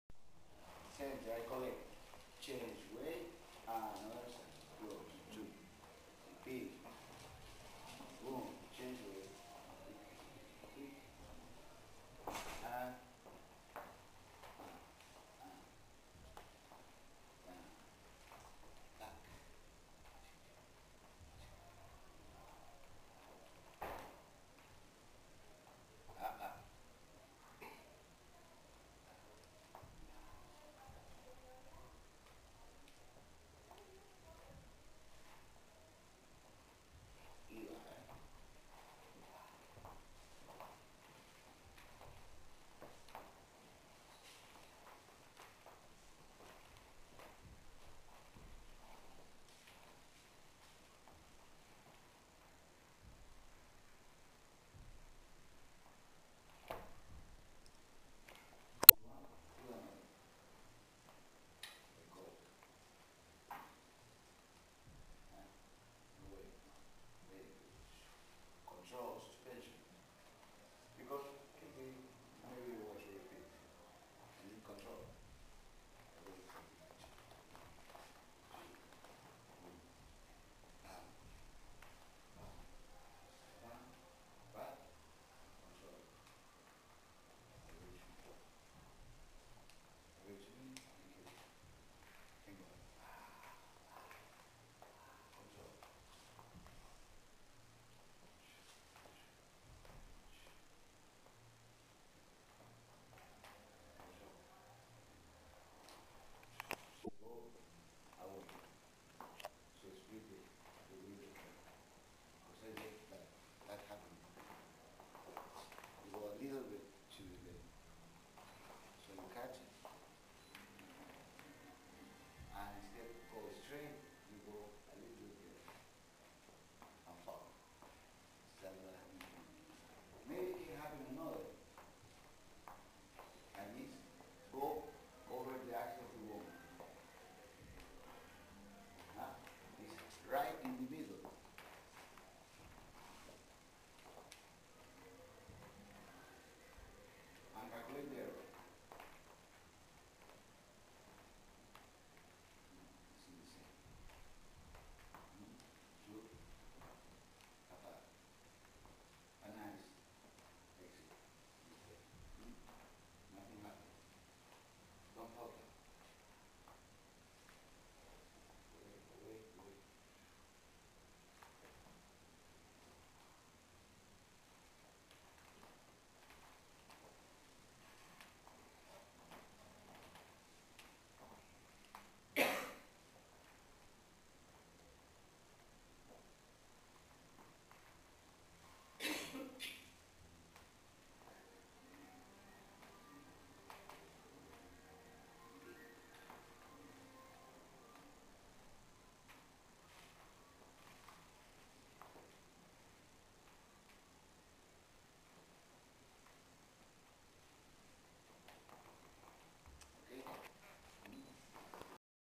Argentine Tango workshop:Pulpo - Enganches (2)
Norberto "El Pulpo (the Octopus)" Esbrez teaches a workshop on enganches & ganchos. New York City. April 10, 2009